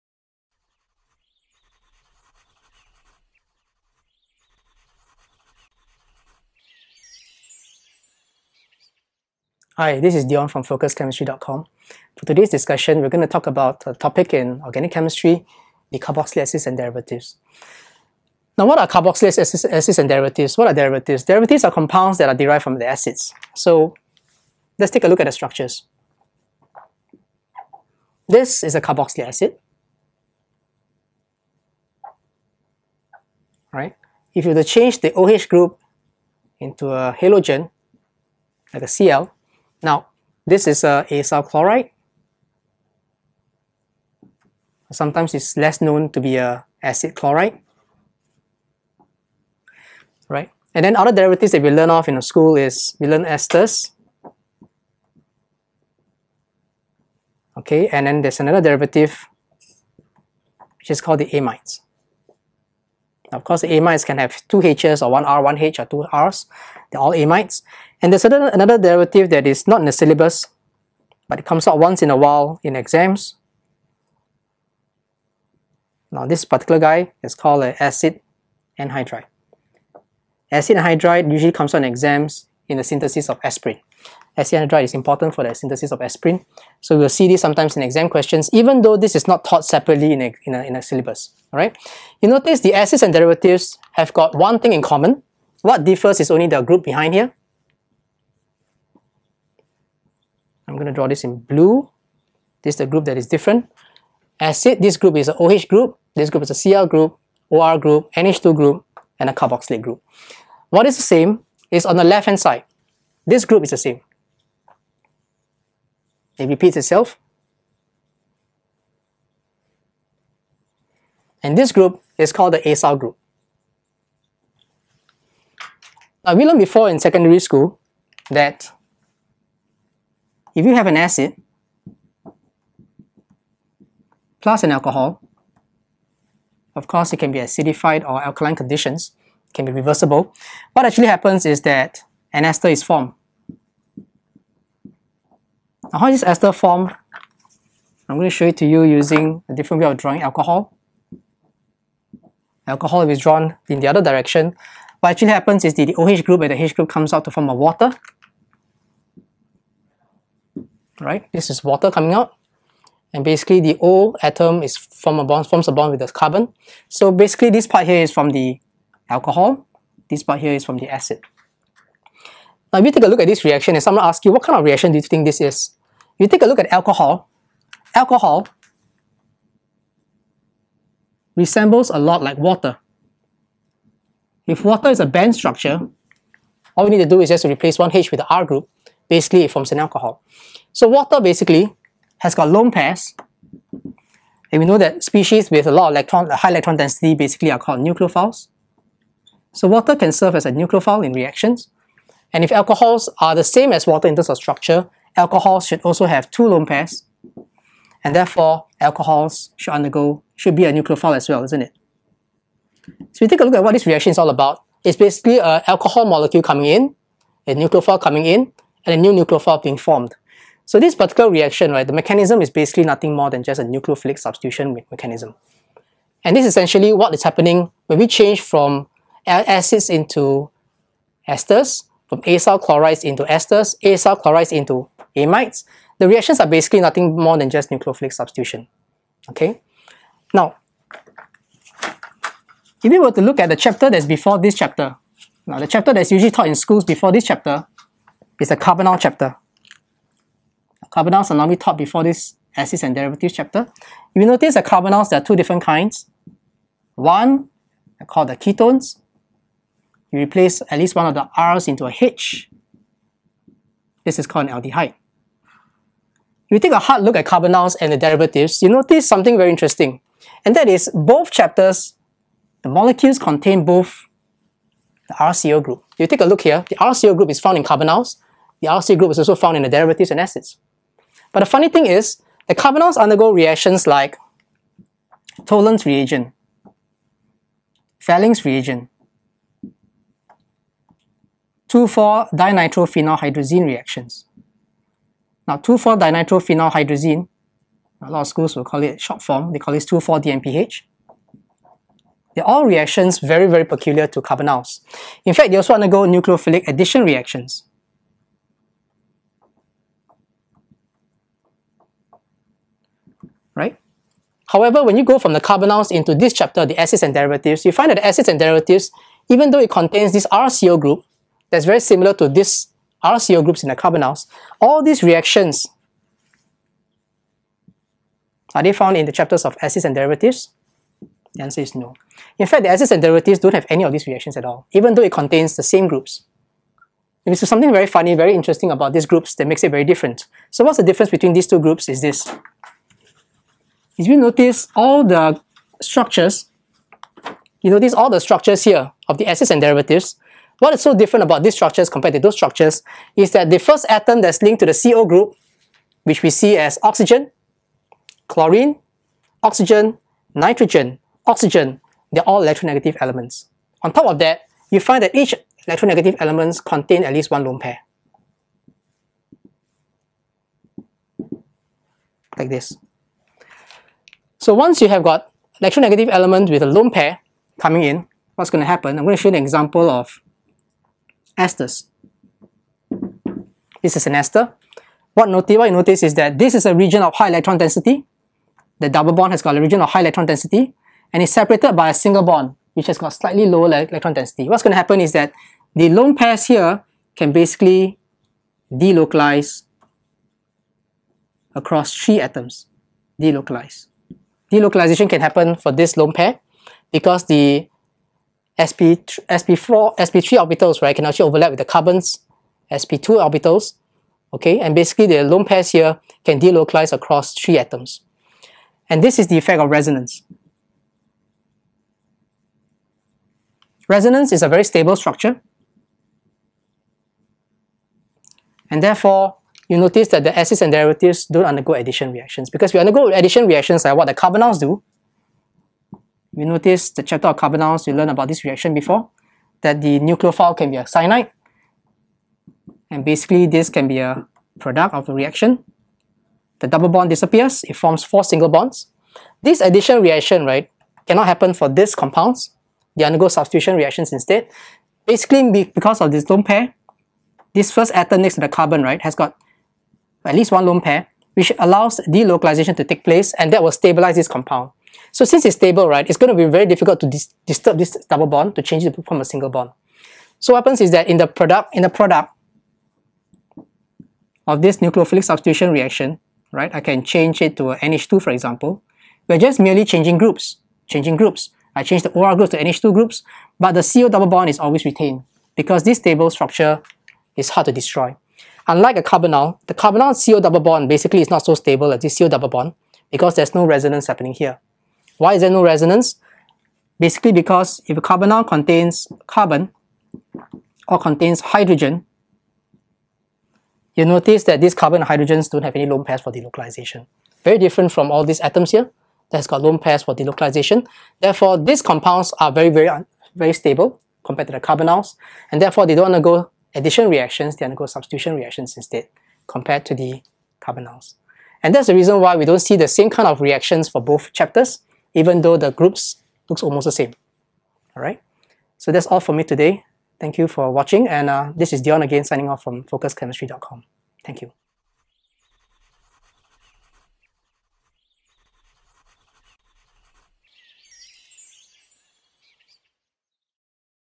A levels Chemistry - Carboxylic Acids and Derivatives
- H2 A level Chemistry : Carboxylic Acids and Derivatives - An interesting feature of its reactions (Nucleophilic Substitution) which makes it different from the Carbonyls (Nucleophilic addition) Part 1